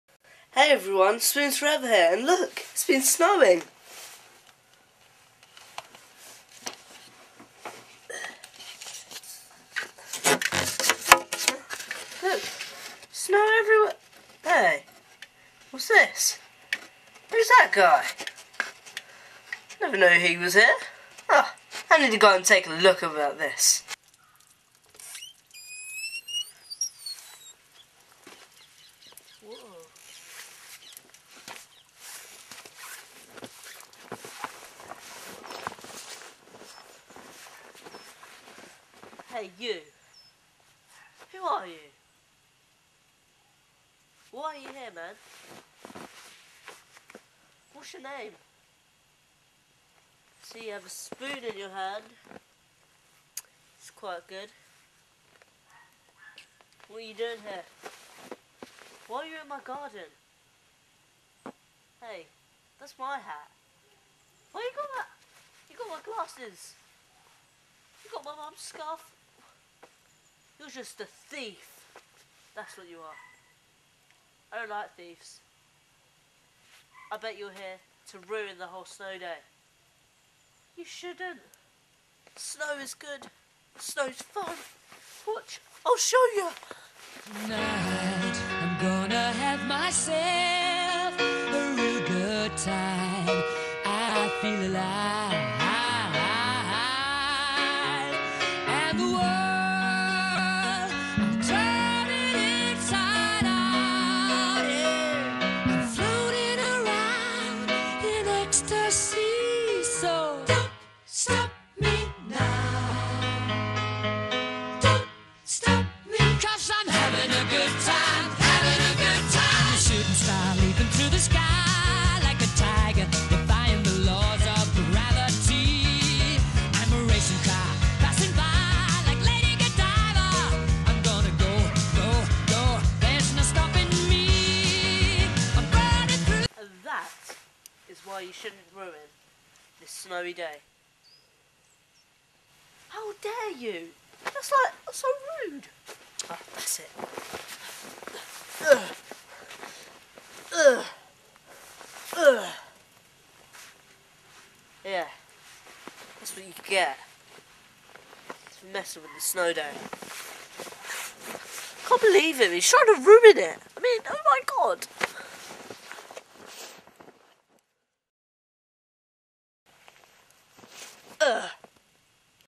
Snow Day | Spoons4evr
It was snowy and everyone was having fun. However, NOONEY the Snowman wasn't having any of it.